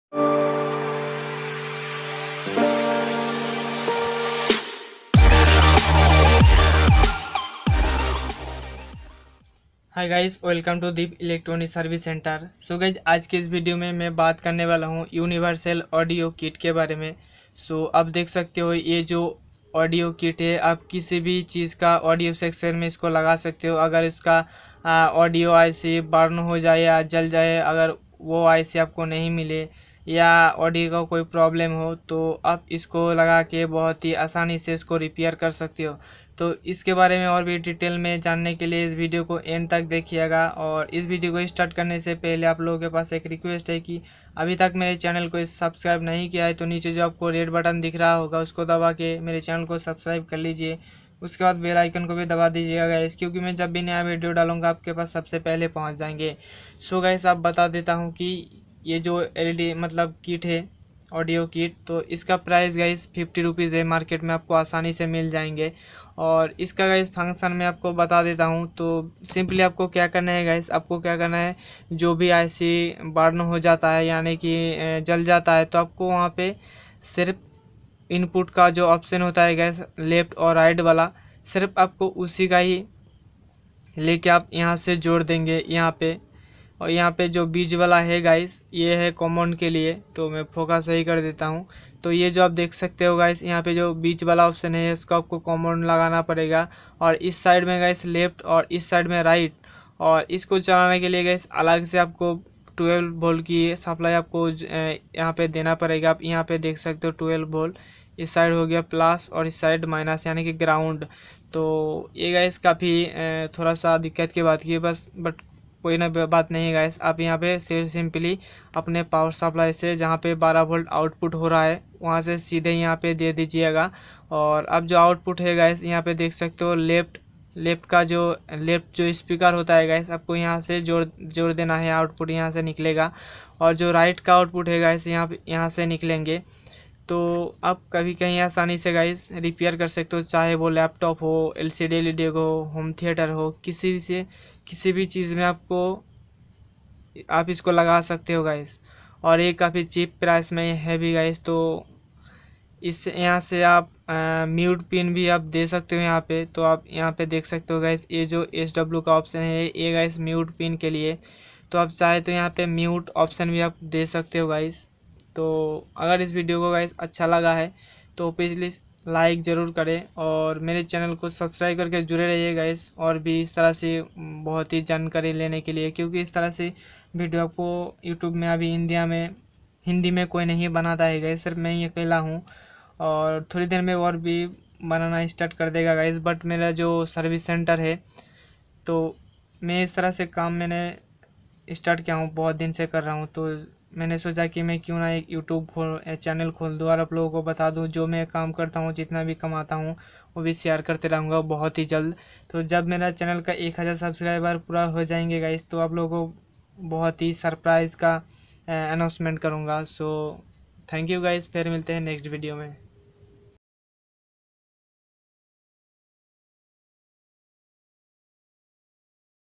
Universal audio kit use in any audio section
Universal audio kit use in any audio section | How to repair no sound problem

About this Video:
In this video I'm telling how to use universal audio kit in any audio section after damage Audio section, if you don't see then see this Video.

About this channel:
I'm uploading video about my own work like electronics gagets repairing video(Laptop, Desktop motherboard, LCD/LED TV, etc repairing)